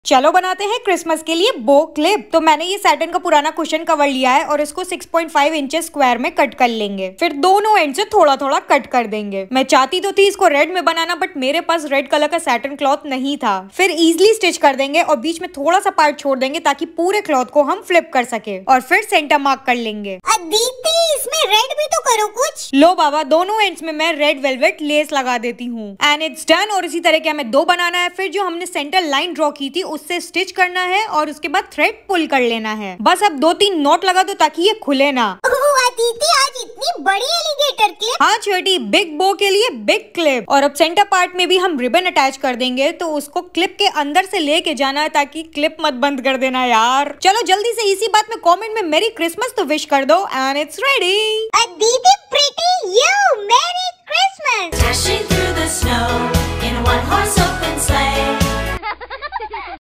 DIY Bow Clip 😱
I'm back with fun DIY. This Christmas Bow Clip is so easy to make and I personally enjoyed the process. Do recreate and share your creation with me on my Instagram Page and get featured.